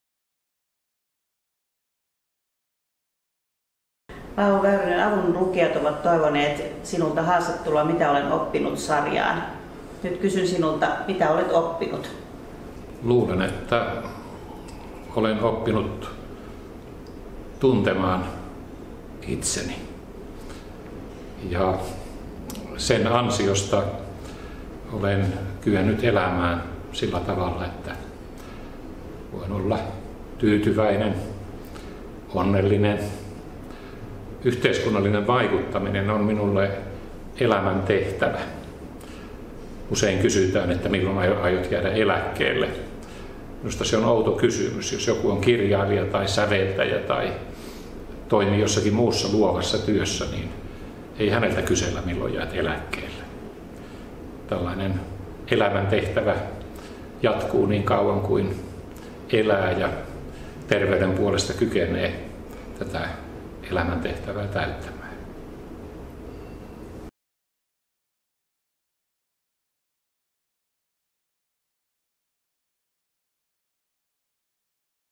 Paavo Väyrynen, mitä olet oppinut?
Paavo Väyrynen vastaa kysymykseen, mitä olet oppinut. Konkaripoliitikko Paavo Väyrynen on katunut vain yhtä asiaa – ”Suomen, Pohjolan ja Euroopan kehitys olisi saanut toisen suunnan”

Tilaa Avun YouTube-kanava ja muista tykätä videosta, jos pidit siitä!